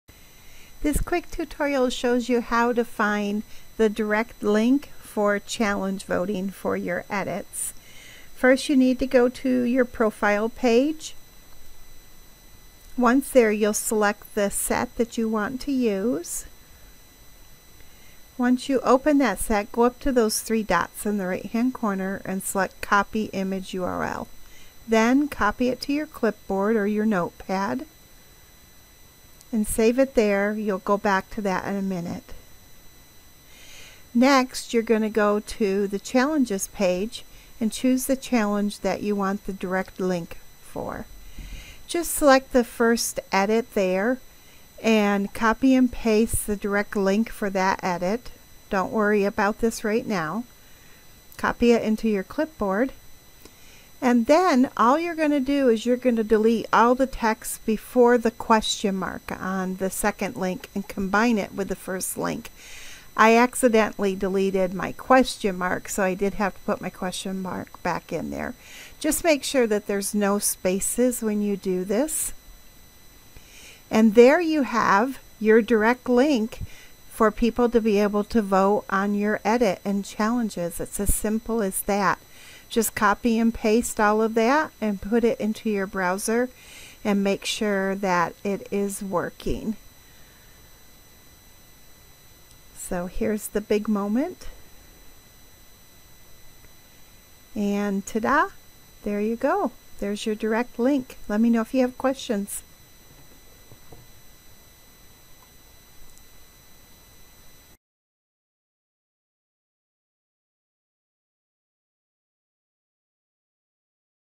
Direct Link for Challenges in PicsArt
How to find the PicsArt direct link for your edits in challenges so people can vote on them.